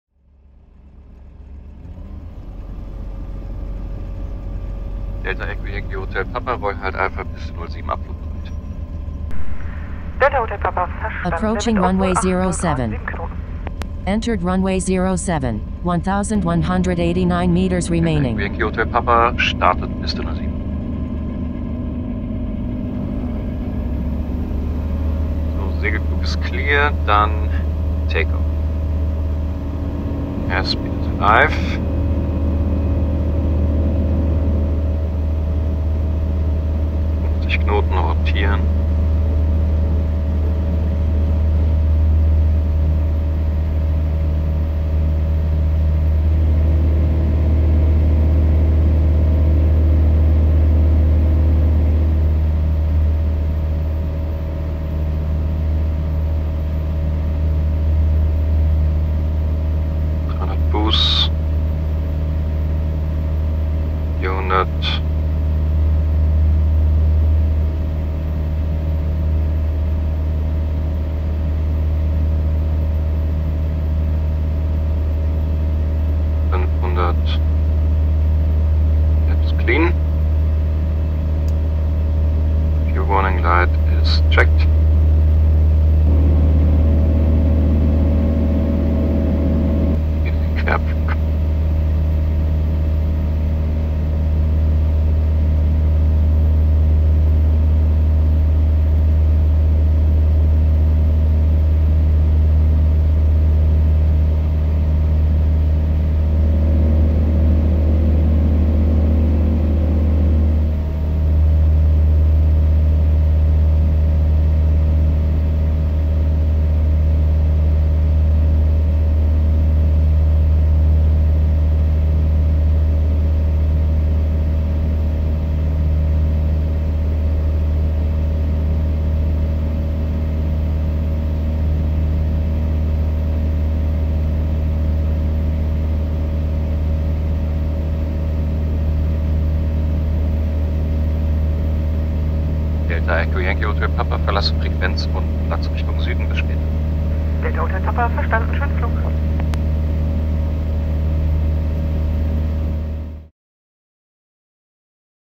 Schönhagen EDAZ VFR Abflug Piste 07
VFR Abflug 07
In diesem Video zeigen wir euch wie der VFR Abflug von der Piste 07 aus dem Cockpit aussieht.
Links zu den Legs:
0:23 Start Piste 07
1:32 Eindrehen in den rechten Querabflug 07
2:20 Verlassen des Platzes